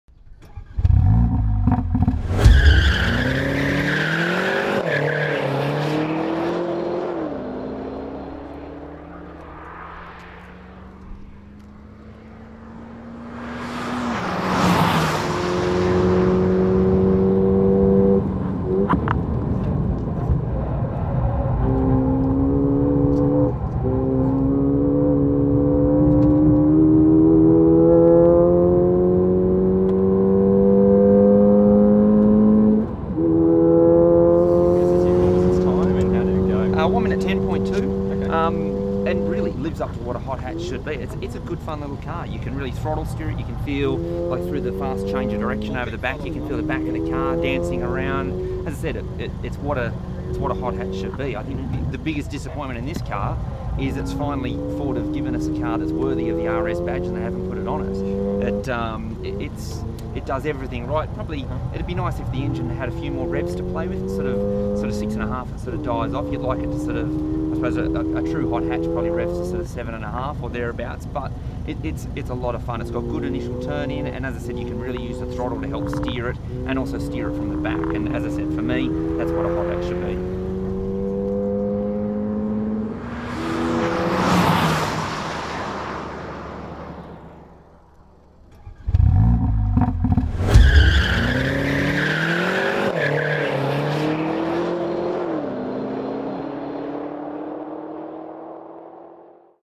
Performance Car of the Year 2013 - Ford Fiesta ST
Ride with Warren Luff as he laps Broadford race circuit in the ford Fiesta ST at MOTOR's Performance Car of the Year 2013.

Fast cars and driving! Official YouTube channel of MOTOR, Australia's favourite performance car magazine since 1954.

Join the conversation